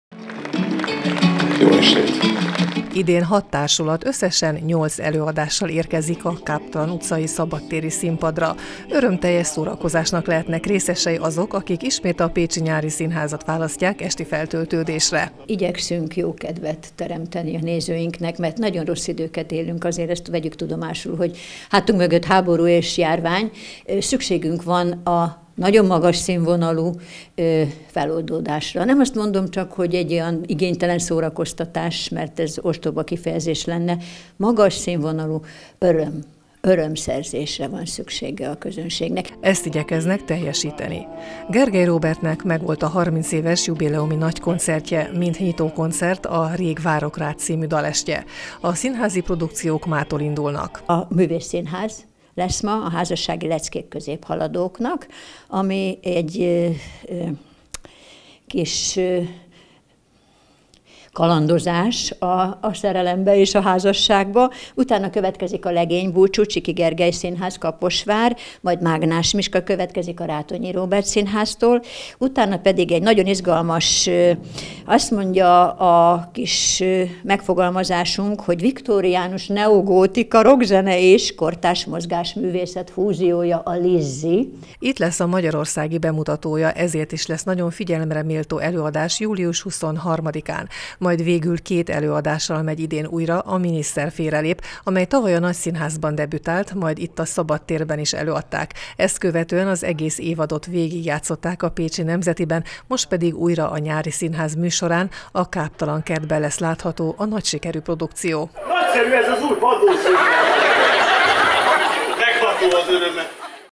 A Pécsi Nyári Színház előadásai
Elkezdődtek a Pécsi Nyári Színház előadásai. Hat társulat 8 produkciója látható júliusban Pécsen a Káptalan Kert szabadtéri színpadán. Vígjáték, musical, bohózat, operett szórakoztatja a nyári esték közönségét. Egy magyarországi bemutató is emeli a rangját a nyári teátrum előadásainak. Telt háznak ígérkeznek a szabadtéri előadásai, de még lehet jegyet kapni. Aki nem használja fel előre megváltott belépőjét, az adja át másnak - kérik a program szervezői.